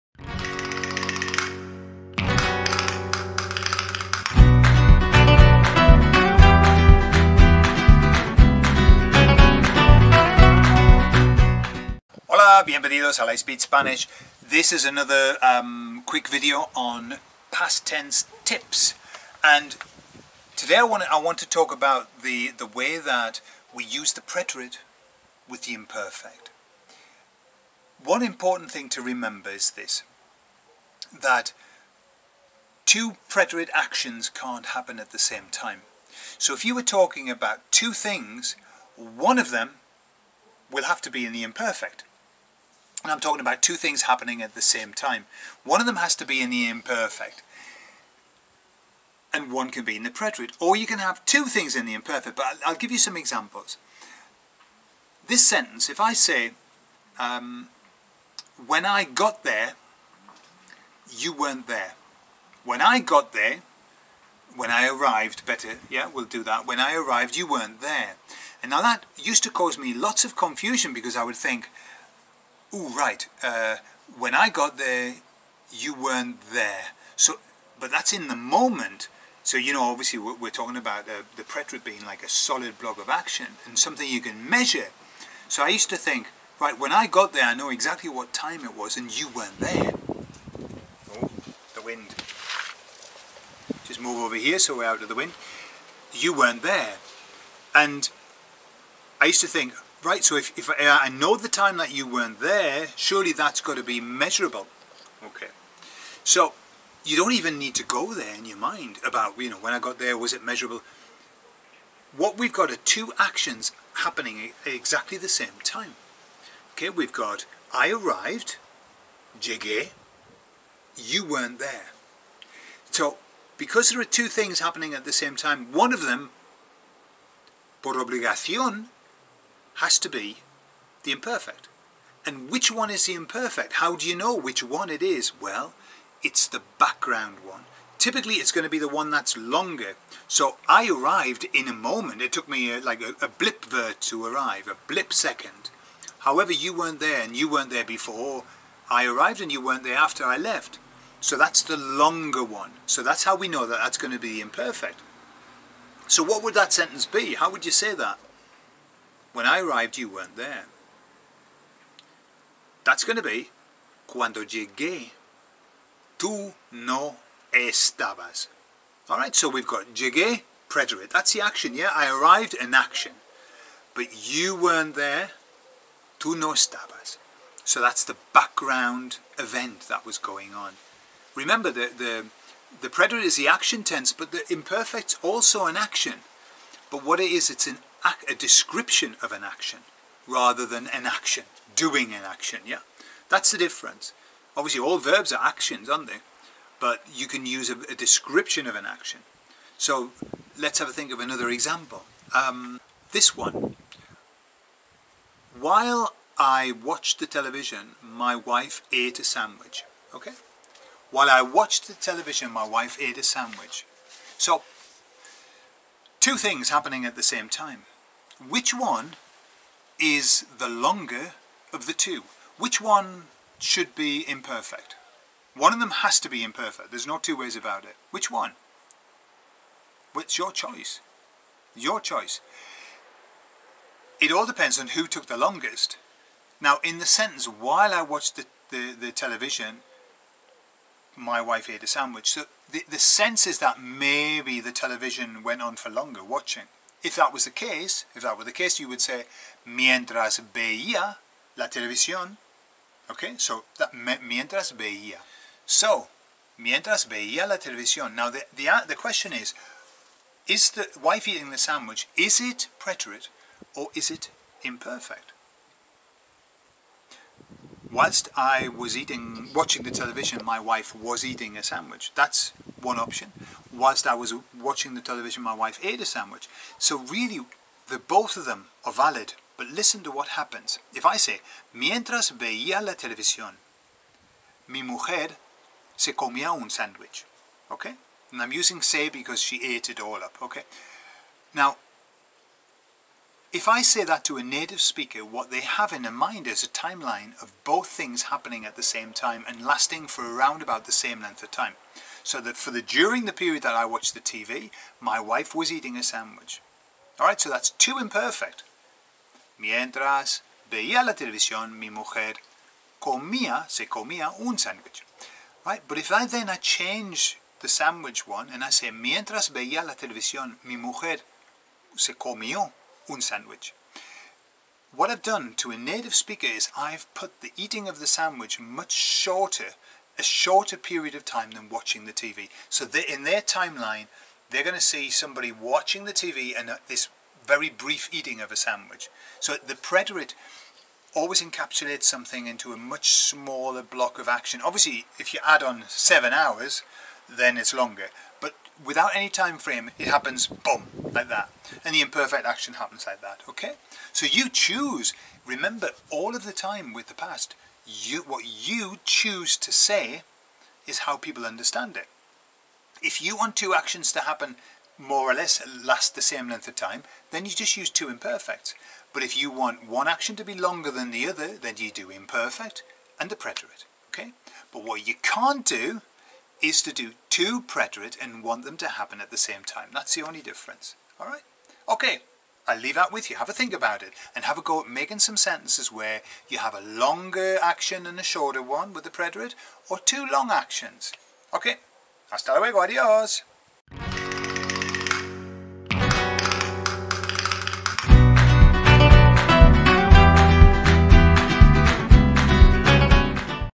Spanish Past Tense Tips 3 LightSpeed Spanish
we are pleased to offer you some great tips on the Preterite and Imperfect Spanish Past tenses.